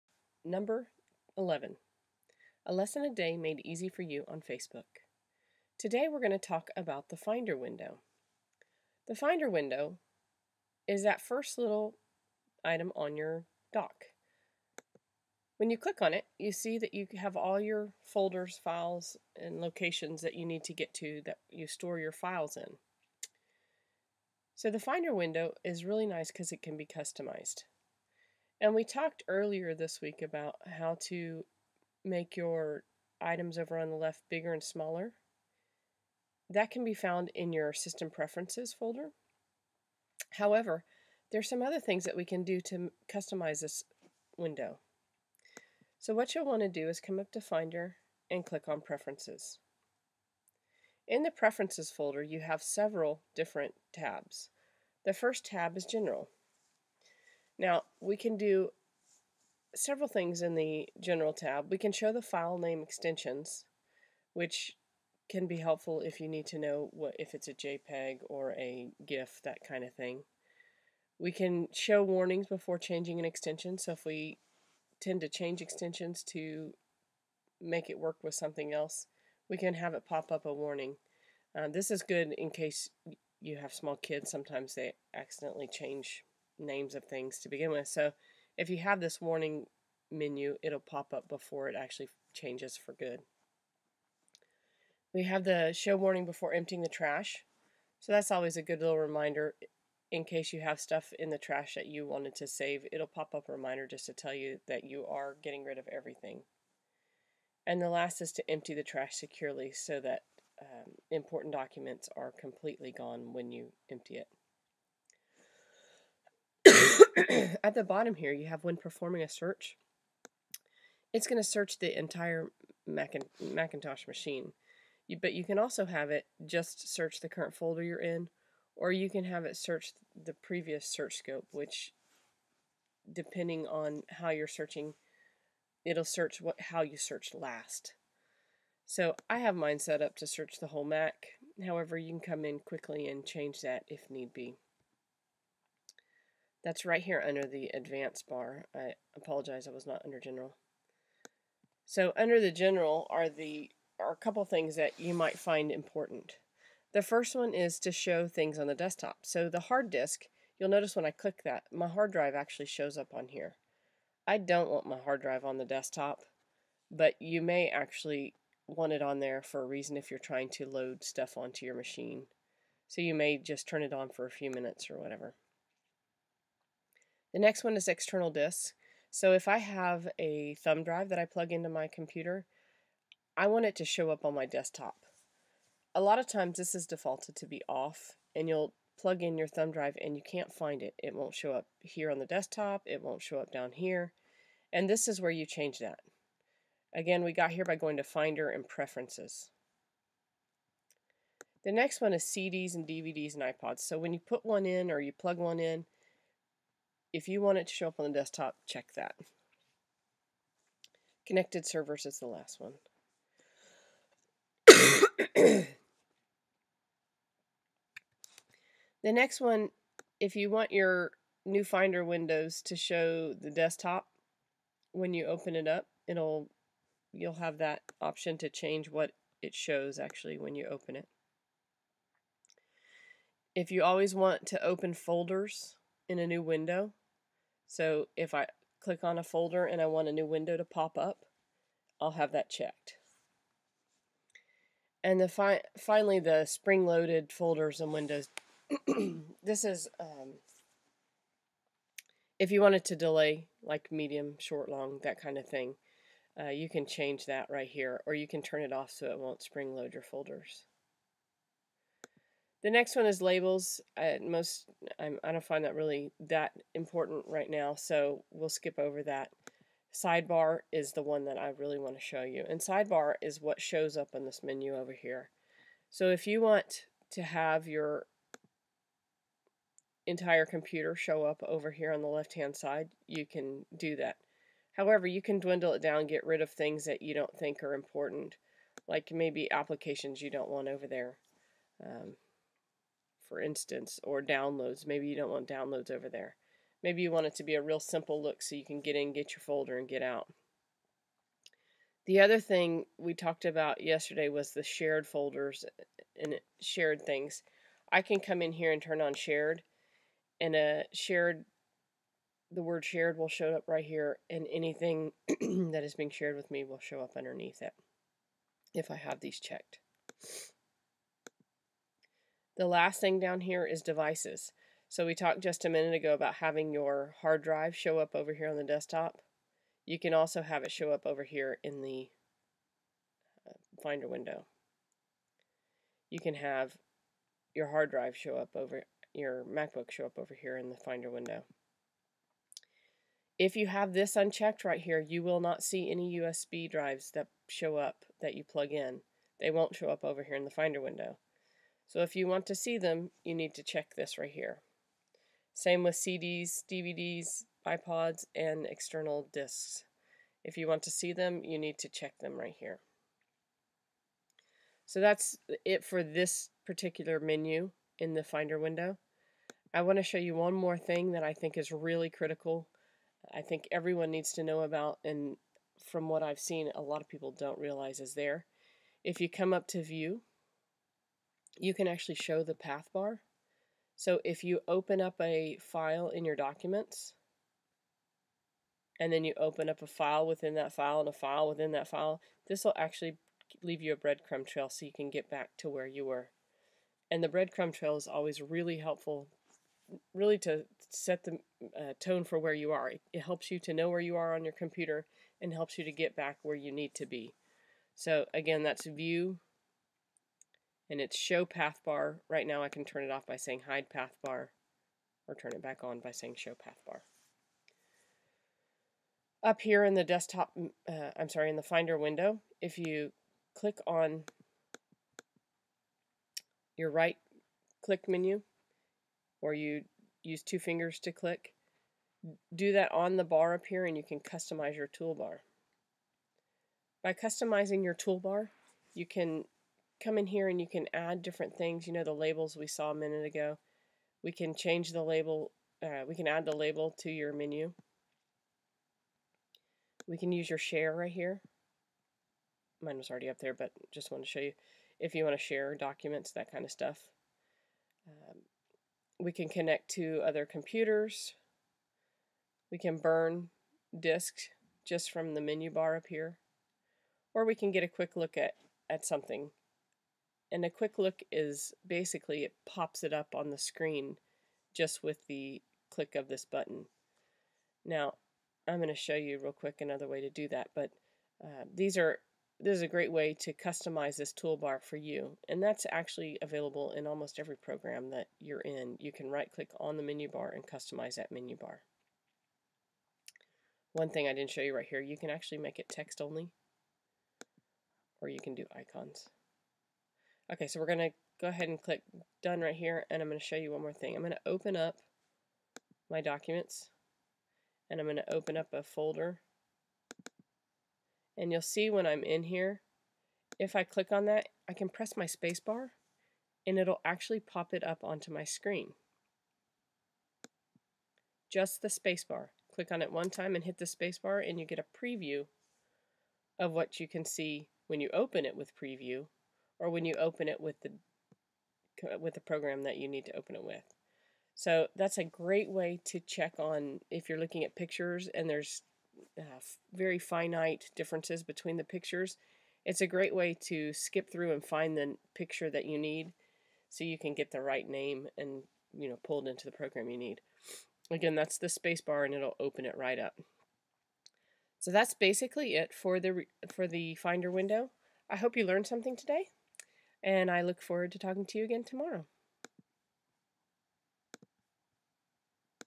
Macbook Lessons #11 Finder Window
Have you ever been unable to find your thumb drive? Or, have you ever wanted to see a breadcrumb trail in your finder window to see where a file is located? Today's tutorial will give you these tricks plus a couple more unknown secrets to your macbook. I promise you will learn something today!